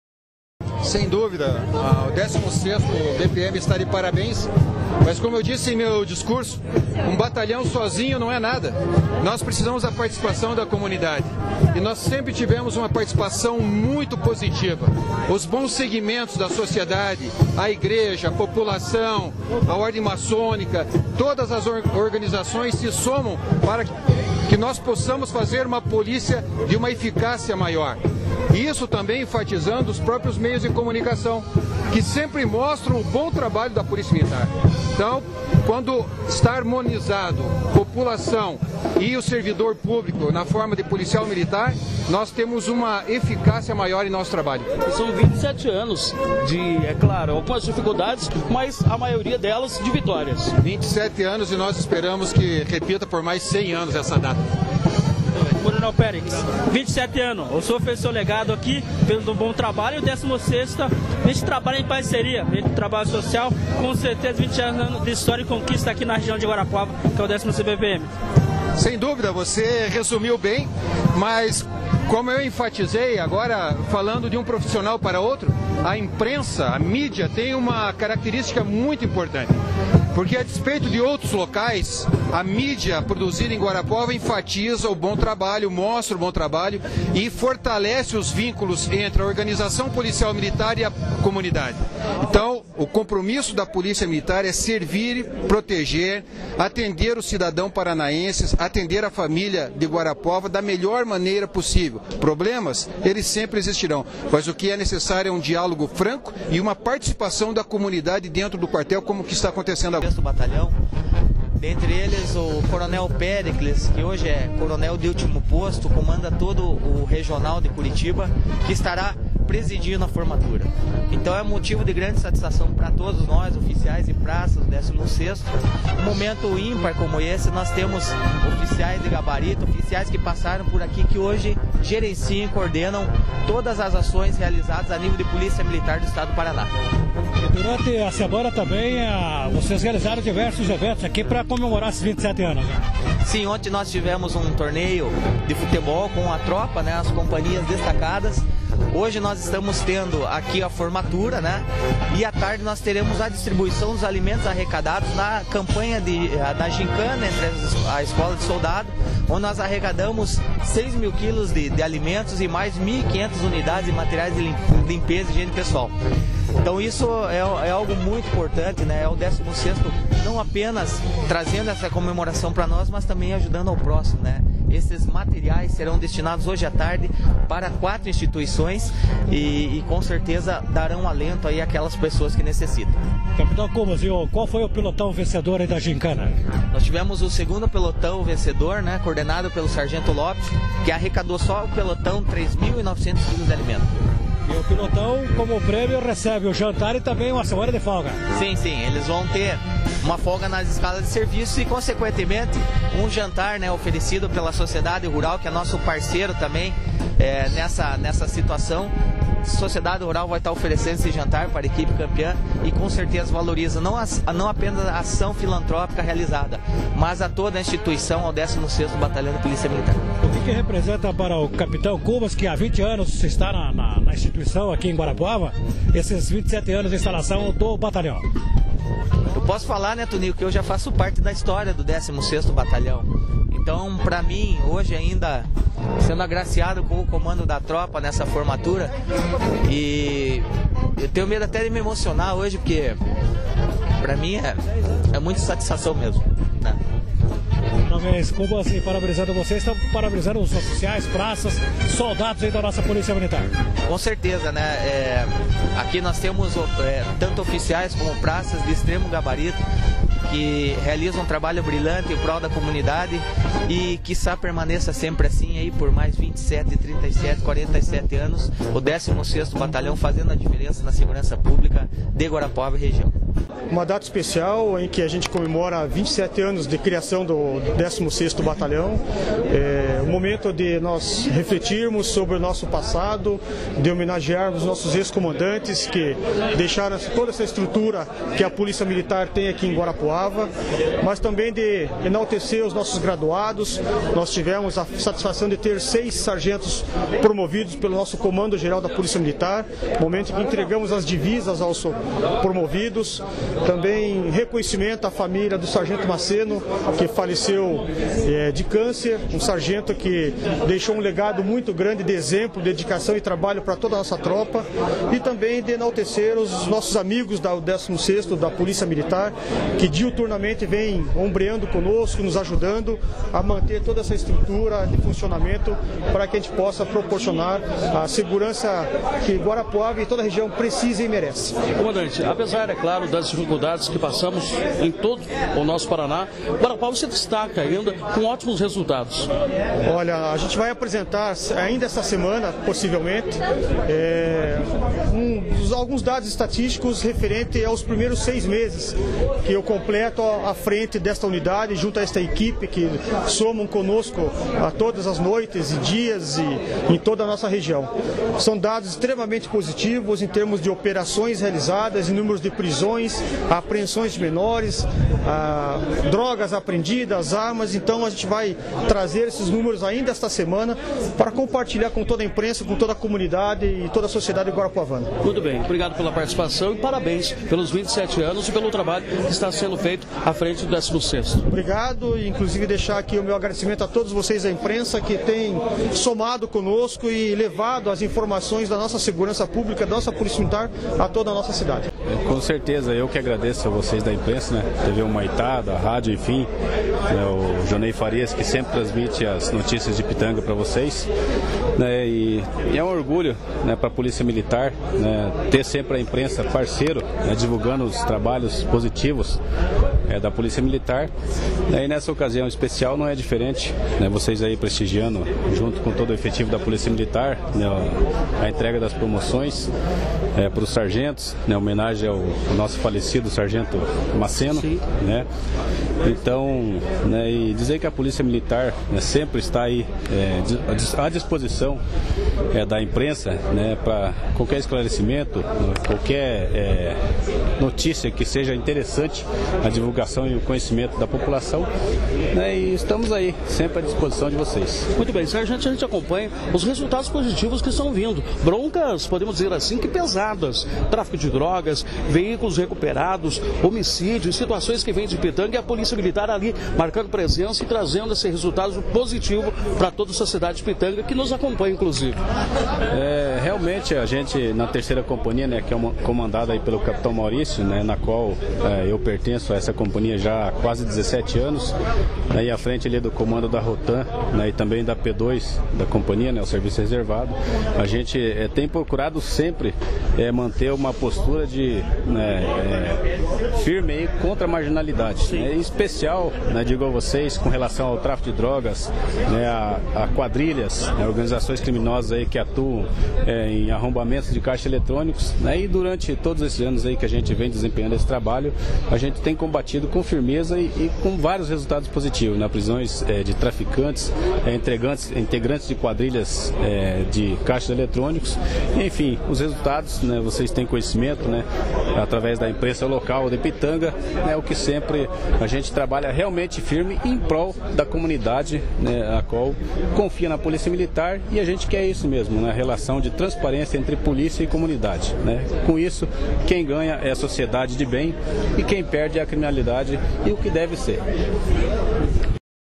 Guarapuava: 27º Aniversario do 16º BPM
Na manhã da última terça-feira (14/06/16), foi realizada na sede do 16º Batalhão de Polícia Militar, em Guarapuava,  solenidade alusiva ao 27º Aniversário do 16º BPM. Acesse Blog//alencarsouza.blogspot.com.br